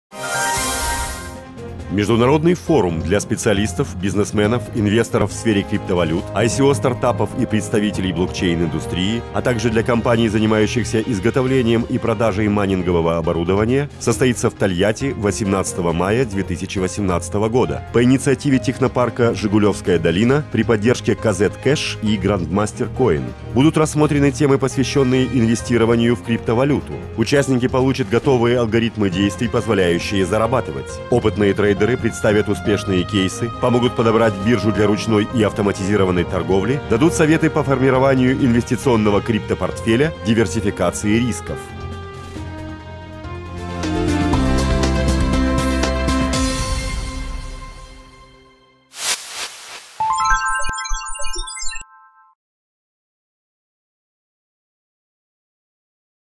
Международный блокчейн форум в Тольятти
Международный форум для специалистов, бизнесменов, инвесторов в сфере криптовалют, ICO-стартапов и представителей блокчейн-индустрии, а также для компаний, занимающихся изготовлением и продажей майнингового оборудования, состоится в Тольятти 18 мая 2018 года по инициативе Технопарка “Жигулевская долина” при поддержке KZ Cash и Grandmaster Coin. Будут рассмотрены темы, посвященные инвестированию в криптовалюту. Участники получат готовые алгоритмы действий, позволяющие зарабатывать. Опытные трейдеры представят успешные кейсы, помогут подобрать биржу для ручной и автоматизированной торговли, дадут советы по формированию инвестиционного криптопортфеля, диверсификации рисков.